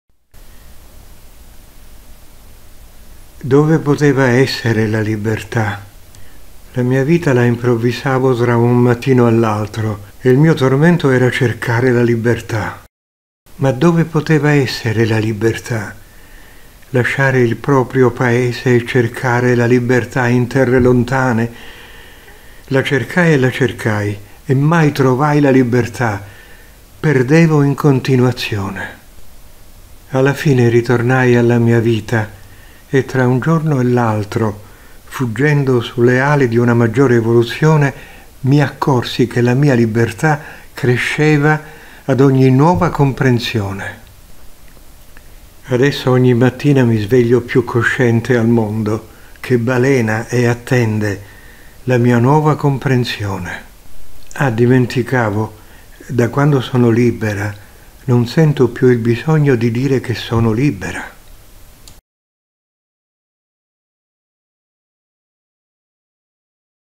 Evolvenza Dove poteva essere la libertà
Si pendant quelques minutes par jour nous prenions en considération la diversité de chaque personne avec laquelle nous sommes en contact, des parents aux amis de football, des enseignants aux collègues de travail, et si nous nous mettions à la place de chacun d'eux, nous aurions moins vision incomplète de notre existence, car nous accepterions tous les points de vue.